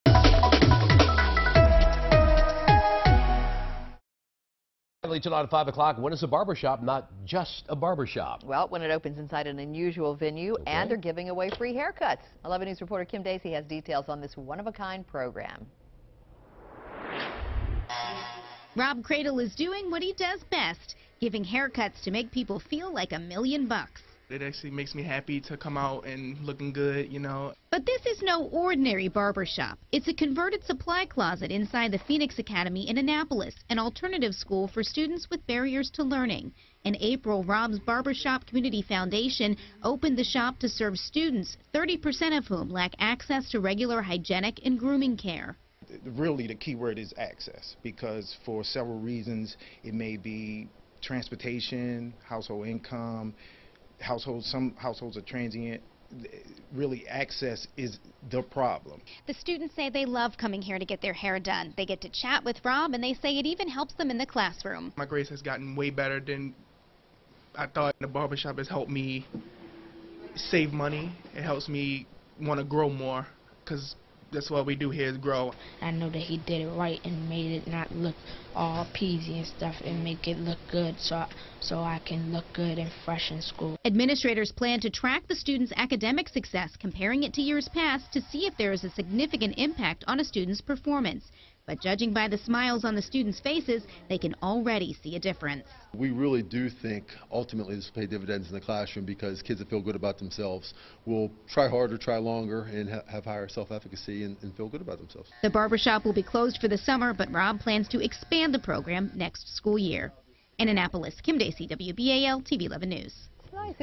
Haircuts help disadvantaged youth in Annapolis
When is a barbershop not just a barbershop? When it opens inside an unusual venue to give free haircuts.